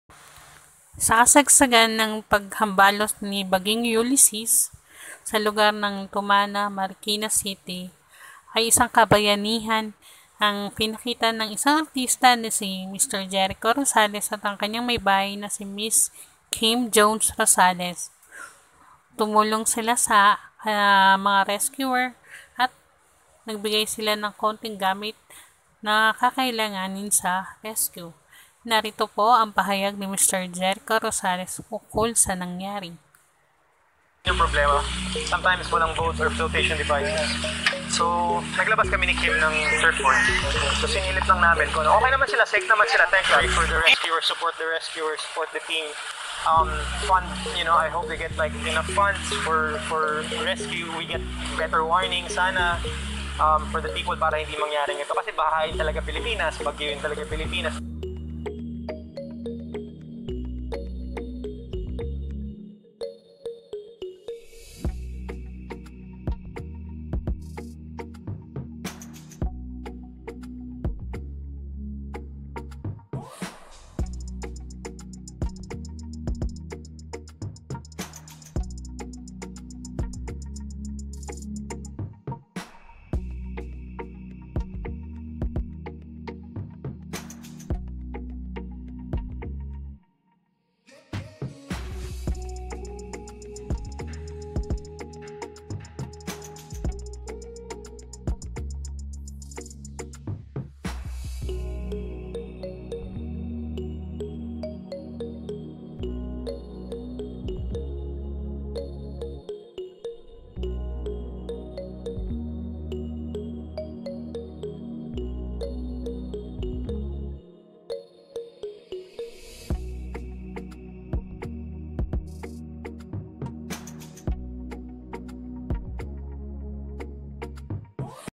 JERICHO ROSALES AT MISIS NA SI KIM JONES TUMULONG SA MGA NASALANTA SA MARIKINA
Actor Jericho Rosales and Wife Kim Jones-Rosales Ay Tumulong Magrescue sa mga Biktima ng Bagyong Ulysses sa Marikina at nagpahiram ng mga kagamitan para sa pagsagip